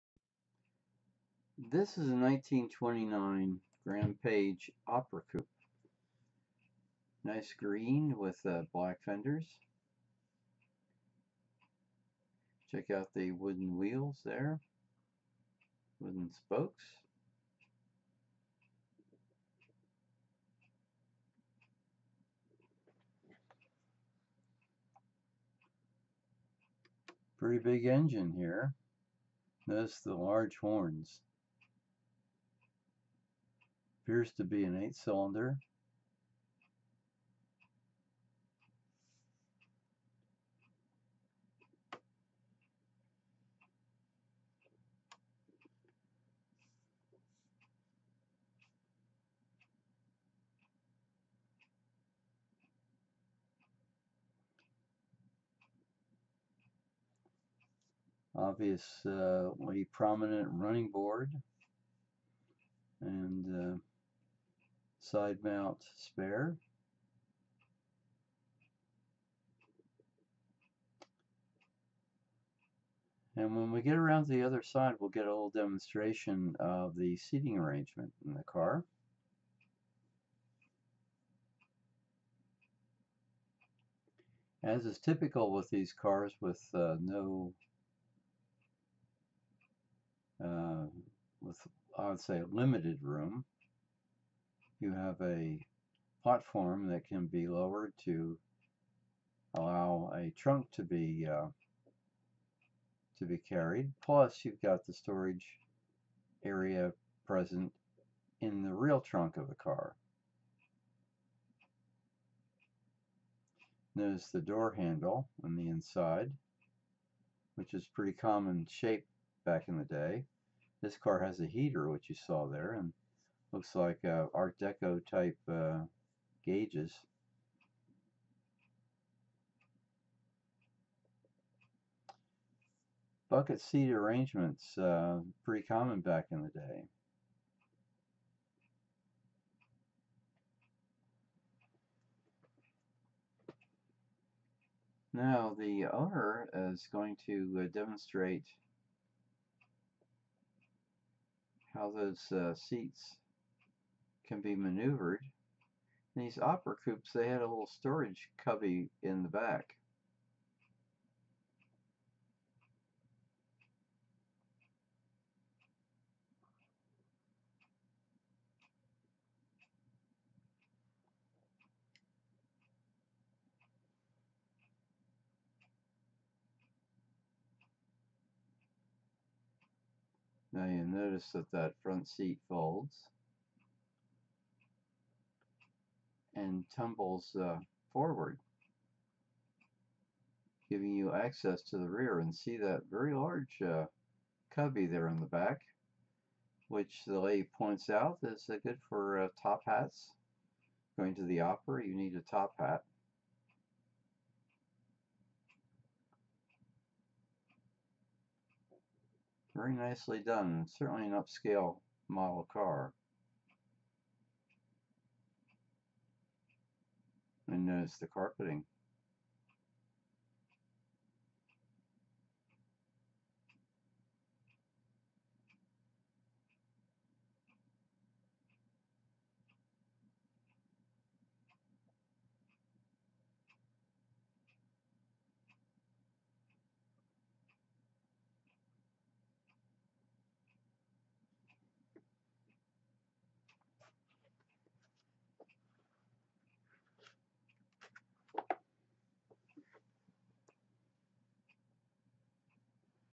1929 Graham Paige Opera Coupe GrnBlk Ocala022517
Just before the Depression thinned the automobile ranks, there were lots of upscale cars like this Graham Paige to give the owners a touch of luxury.  This impressive coupe would get the owner to an opera in style!

Show less